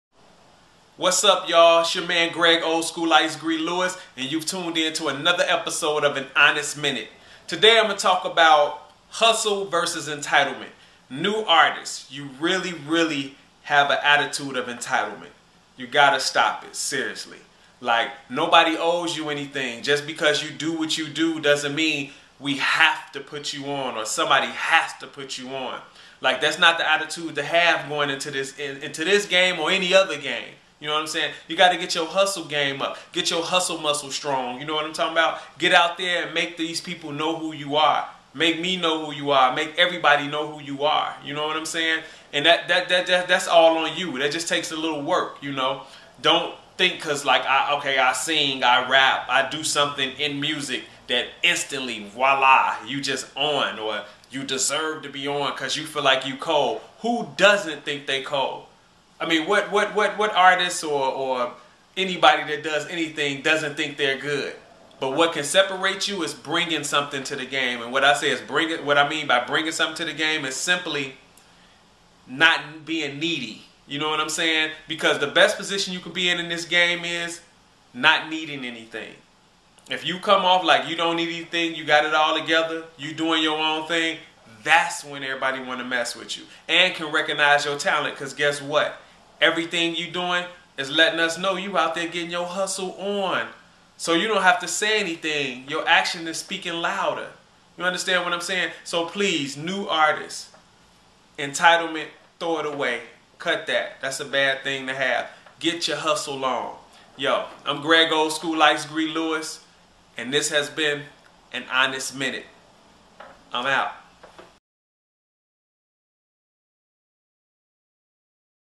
An Honest Minute Episode 4
In this episode of An Honest Minute Greg "Olskool Ice-Gre" Lewis speaks on Hustle vs Entitlement.

Twitter Follow: @Olskool_IceGre